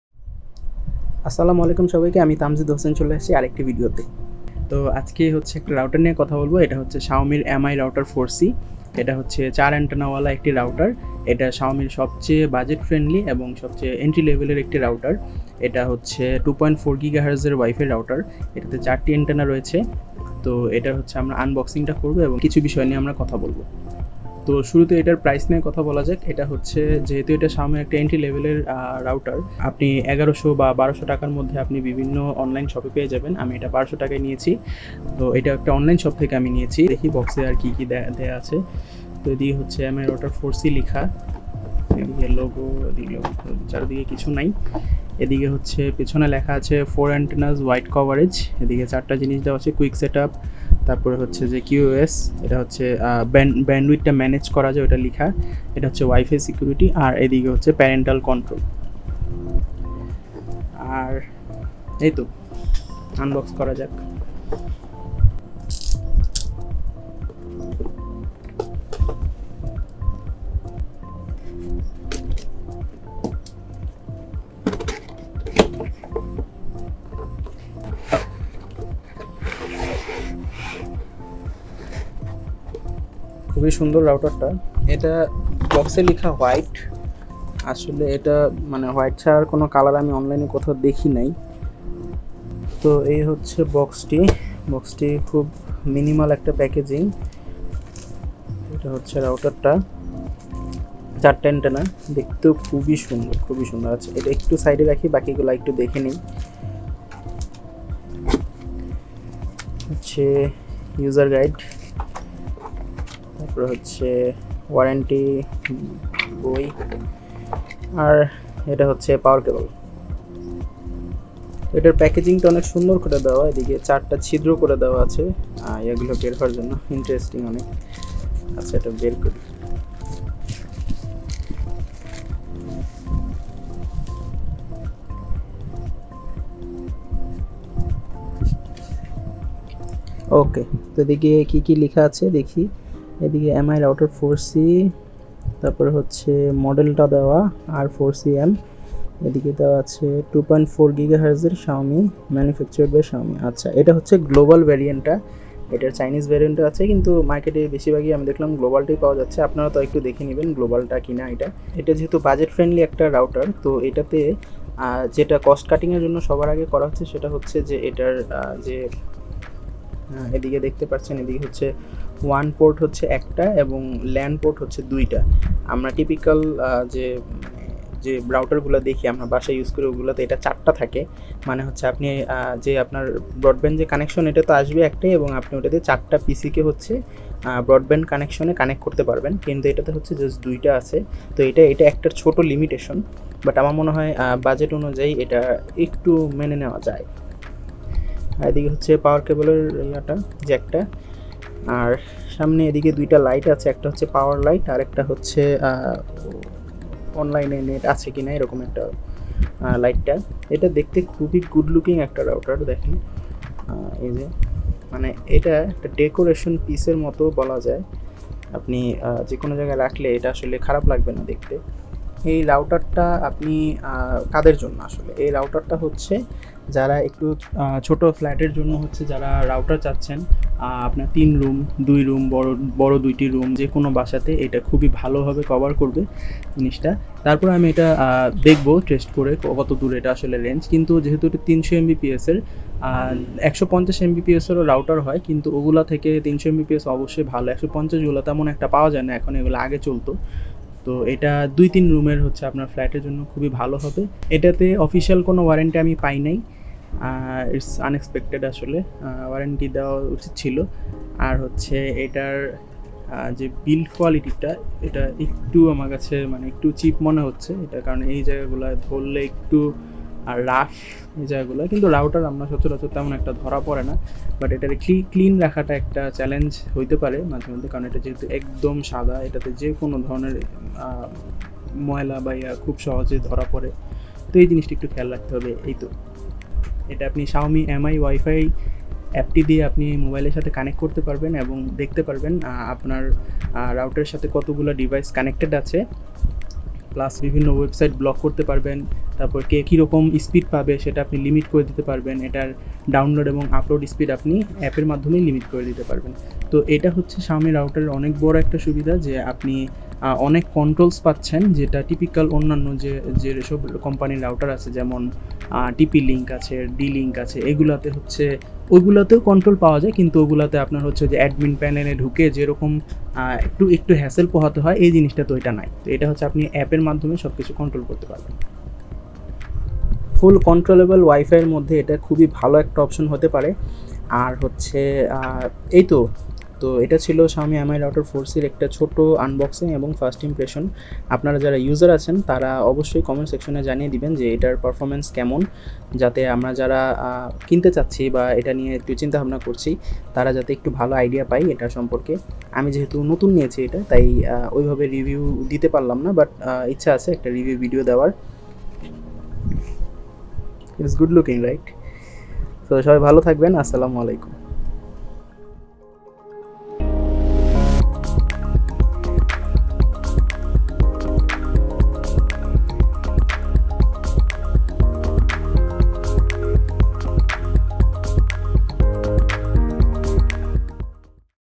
Xiaomi Mi Router 4C Unboxing & First Impression | Pros and Cons
Mi 4C is an entry-level router offering Xiaomi with 300 Mbps speed and good local area coverage. It is good for small to medium houses and offices. It is a simple unboxing and first impression video of Mi 4C to look around.

Price: Around 1200 BDT

Cheers.